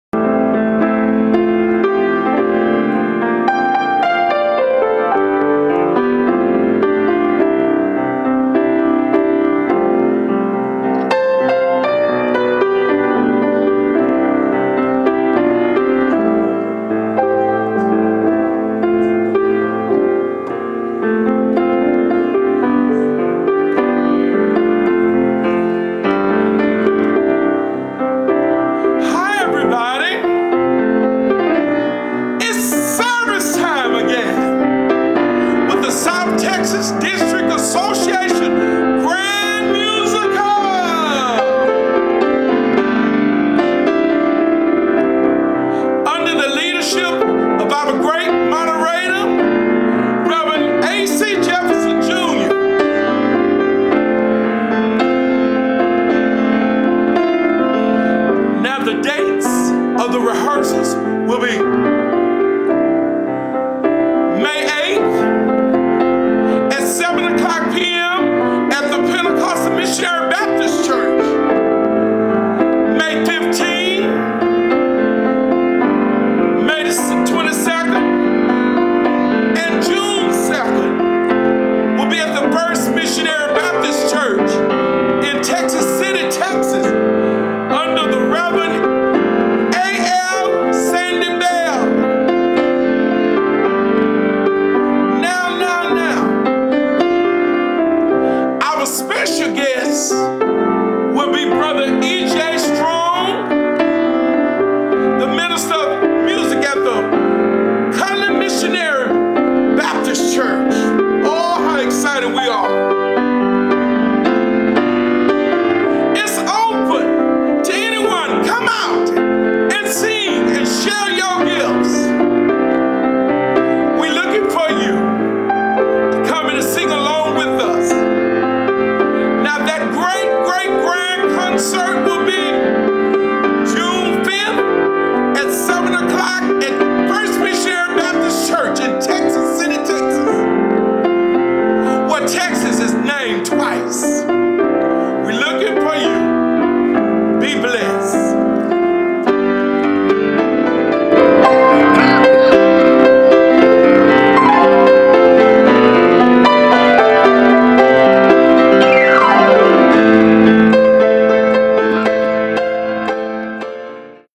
Announcement for the STDA Musical
Announcement for the STDA Musical, June 4th/ Monday 7:00pm. by Boris Trent Williams. Minister of Music at First Baptist Church, Texas City, Texas